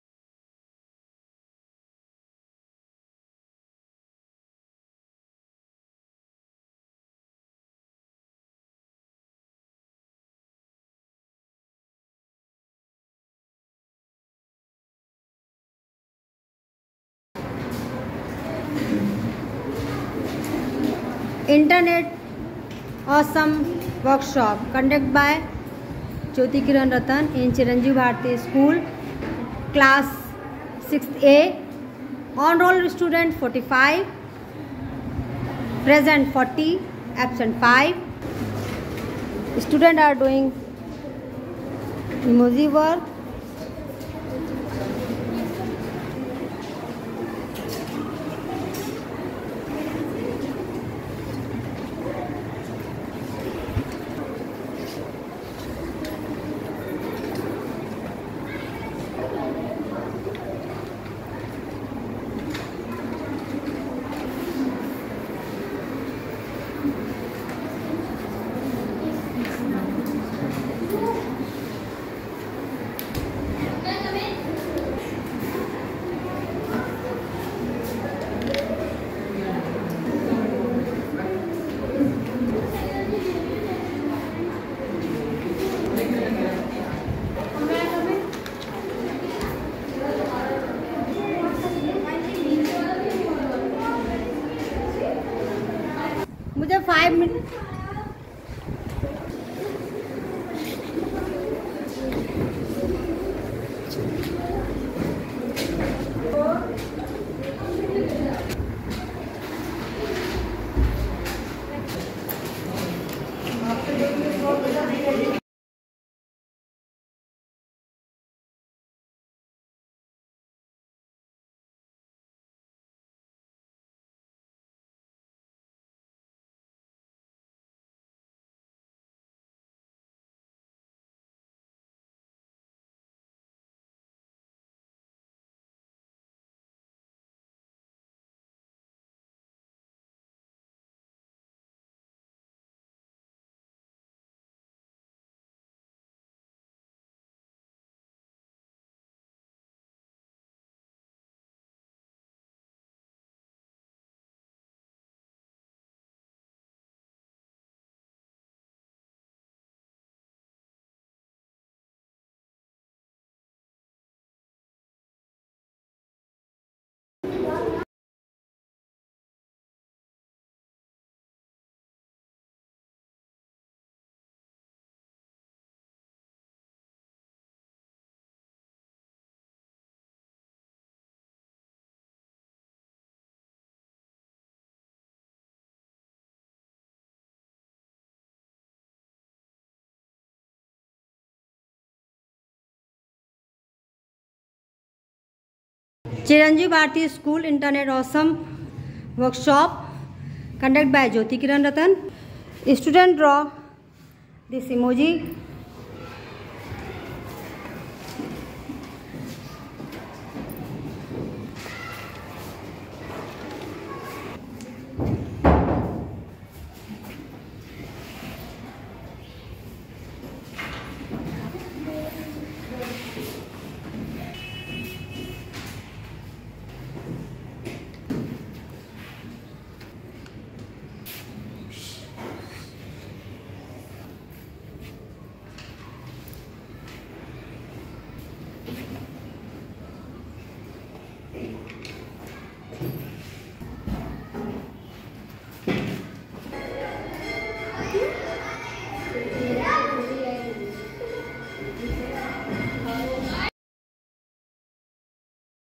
Jyoti Kiran ratan folk dance
internet awesome workshop conduct by Jyoti Kiran ratan in chiiranjiv bharti school sector M AASHIYANA LUCKNOW